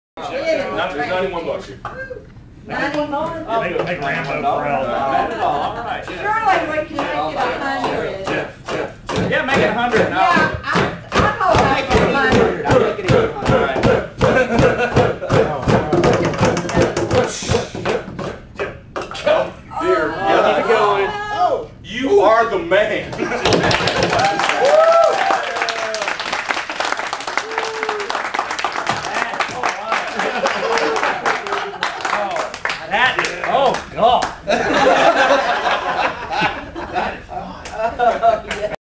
Jones Soda Seahawks edition
One of our co-workers chugging 3/4 of a bottle of Jones Soda Perspiration for charity.